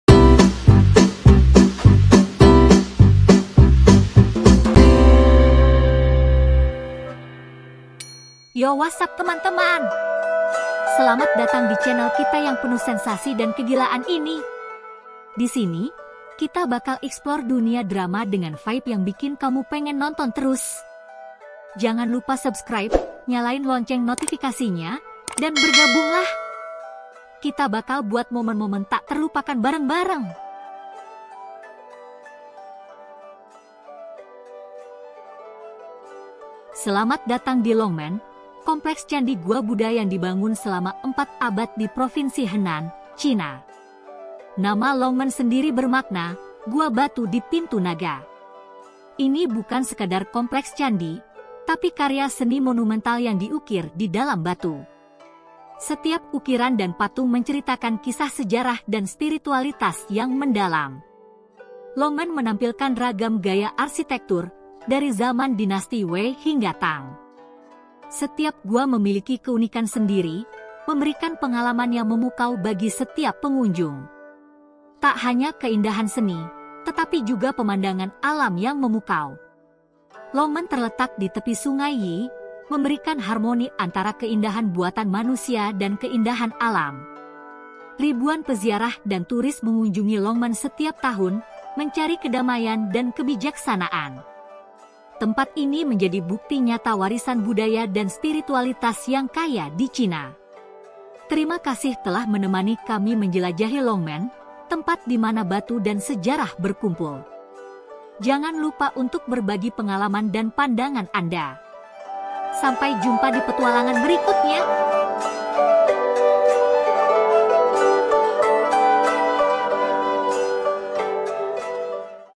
Longmen China
"Selamat datang di Longmen, kompleks candi gua Buddha yang dibangun selama empat abad di Provinsi Henan, China. Nama Longmen sendiri bermakna 'gua batu di Pintu Naga.'"